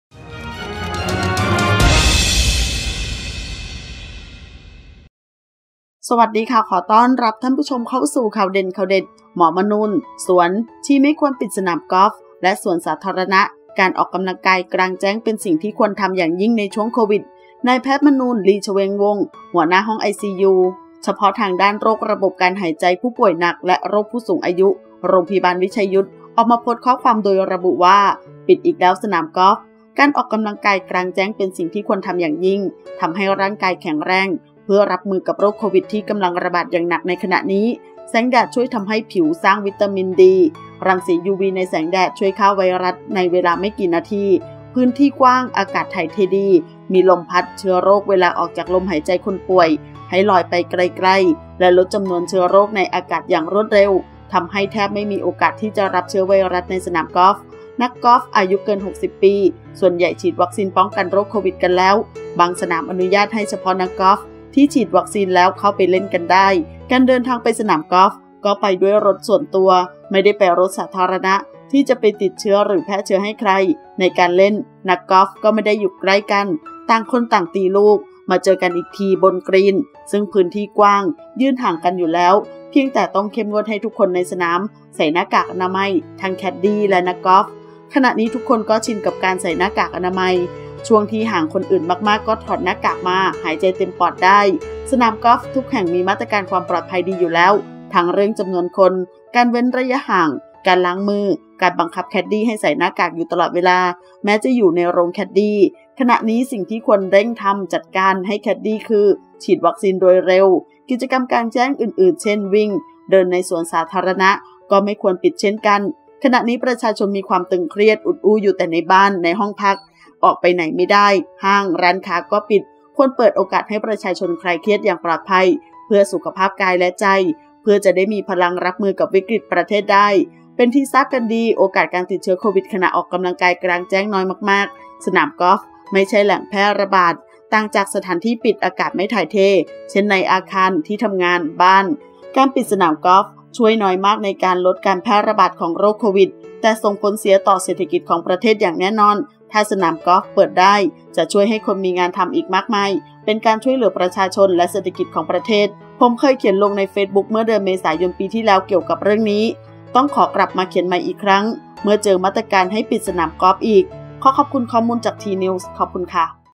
“หมอมนูญ” สวน ชี้ไม่ควรปิดสนามกอล์ฟและสวนสาธารณะ การออกกำลังกายกลางแจ้งเป็นสิ่งที่ควรทำอย่างยิ่ง
ขอบคุณที่มา :
ขอขอบคุณข้อมูลจาก: